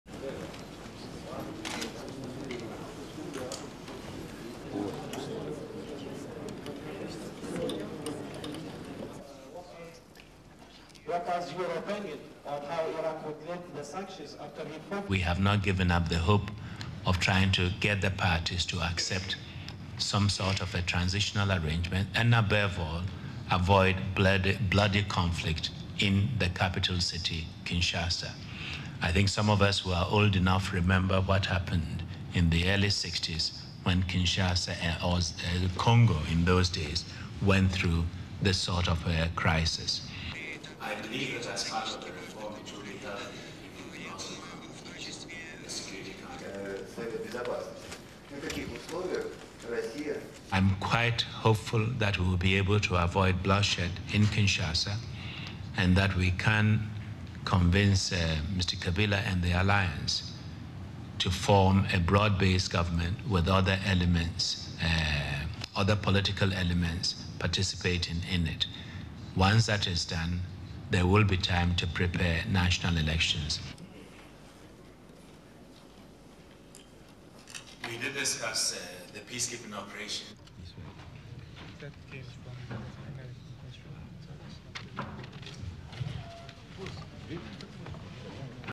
Russia - UN Secretary General Kofi Annan presser
(17 May 1997) T/I 10:49:22

UN Secretary General Kofi Annan called on Saturday (17/05) for a
peaceful transition to democracy in Zaire, adding that he hoped
a bloody conflict could be avoided in the capital, Kinshasa.

SHOWS:

MOSCOW, RUSSIA   17/5

UN Secretary General Kofi Annan sits at head of room;
cutaway camera;
WS presser;
SOT Annan in English: "We have not given up the hope of
trying to get the parties to accept some sort of a transitional
arrangement and therefore avoid a bloody conflict in thre
capital city, Kinshasa. I think some of us are old enough to
remember what happened in the early sixties when Kinshasa was
Congo in those days went through this sort of crisis."
WS presser;
Vs audience:
Annan SOT in English: "I'm quite hopeful we will be able to
avoid bloodshed in Kinshasa and that we can convince Mr Kabila
and the alliance to form a broad-based government with other
political elements participating in it. Once that is done there
will be time to prepare national elections."
Annan stands;
shakes hands at podium;
exits.

RUNS: 1.28